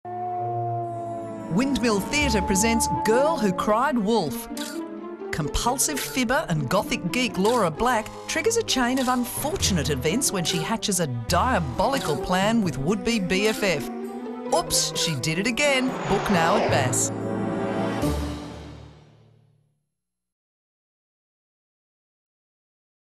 Girl Who Cried Wolf TVC - Windmill Theatre - 2011
Windmill Theatre presents Girl Who Cried Wolf

Written by Angela Betzien

Meet Laura - habitual fibber and high school geek. Then there's Catriona - the popular princess, upset because her jet-setting parents are going to miss her 13th birthday.

Seeing an opportunity to score popularity points, Laura convinces Catriona to lure Mummy and Daddy back from OS. But as soon as their diabolical plan is set in motion an unfortunate series of events begins to unfold in a way that neither of the girls could have ever imagined.

This is a high tech, witty and sharp-tongued satire on the politics of the playground.

Originally produced by Arena Theatre Company with the support of the Sydney Opera House.